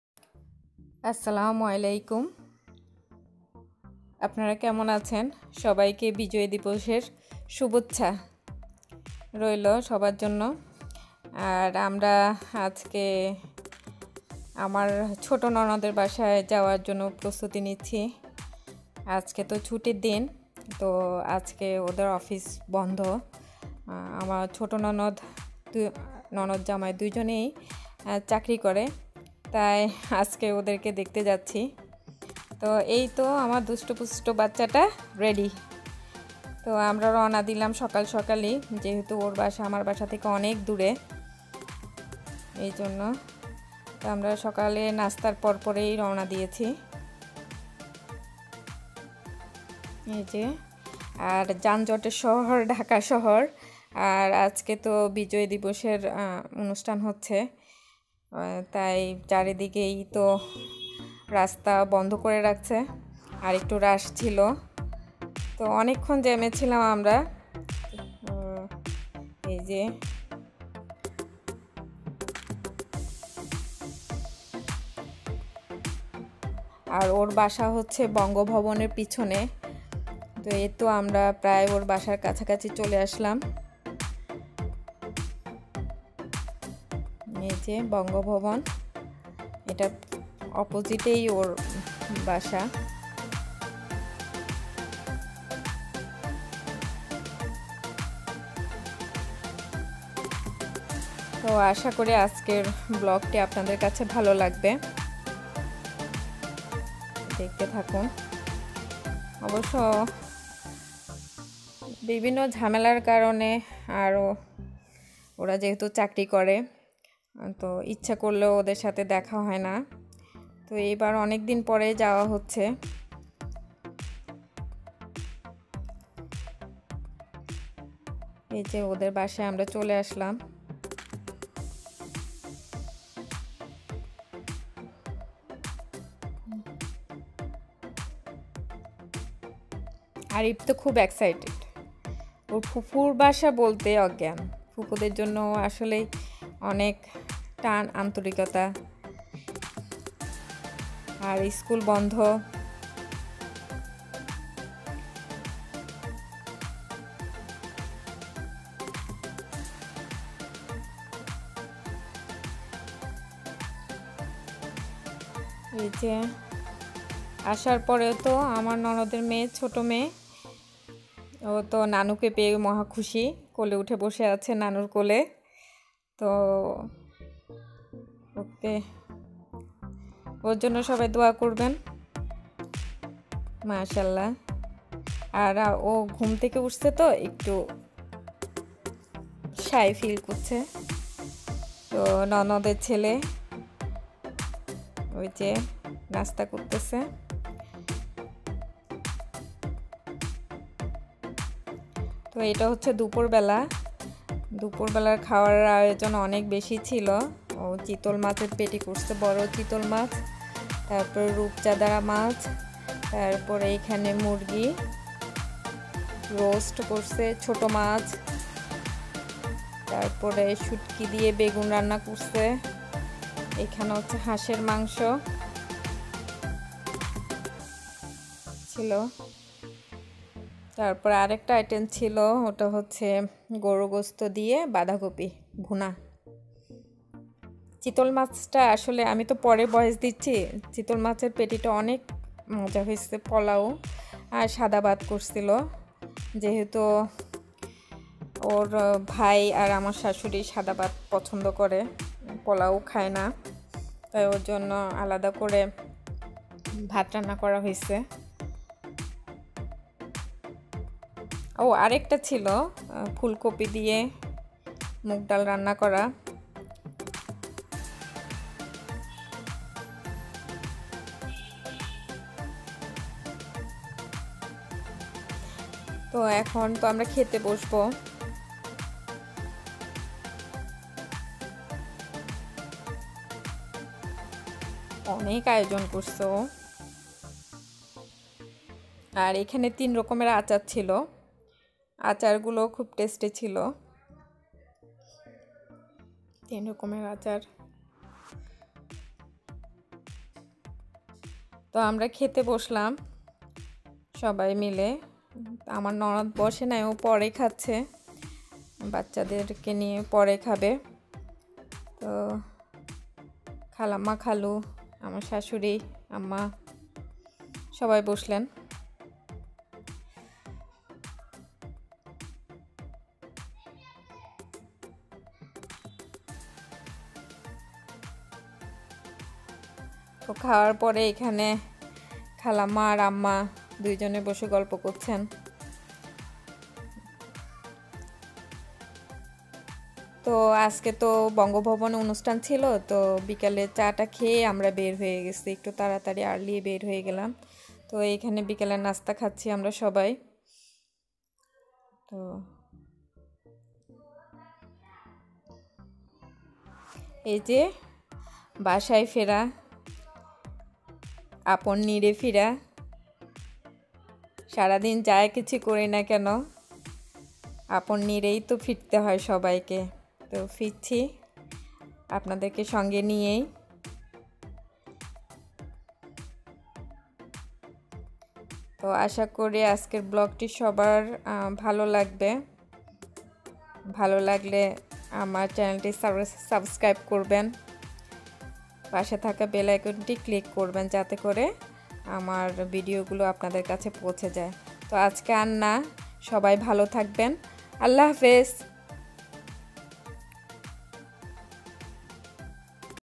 Vlog#17 ছুটির দিনে ননদের বাসায় দাওয়াত খেতে যাওয়া।
১৬.১২.১৯
আসসালামুয়ালাইকুম, সবাই কেমন আছেন। আলহামদুলিল্লাহ আমি ভালো আছি। সবাইকে বিজয় দিবসের শুভেচ্ছা।